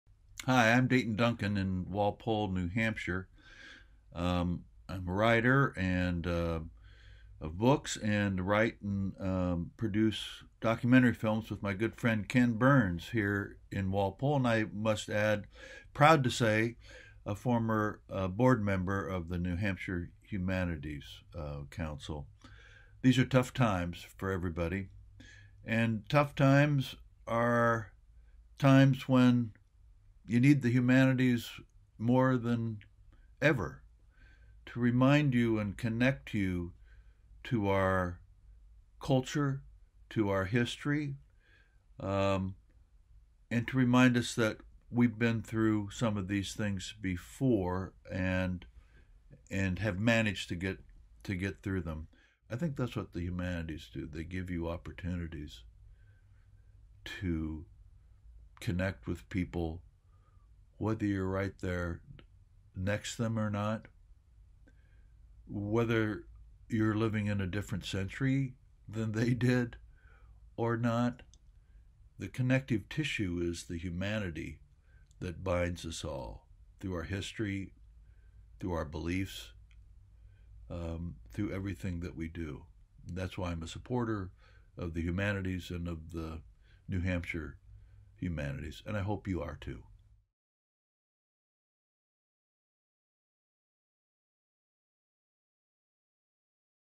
Writer & Producer Dayton Duncan on the Humanities
We were honored to have acclaimed writer & producer Dayton Duncan record this message for us on the importance of the humanities during this difficult time.  The full, unedited version can be viewed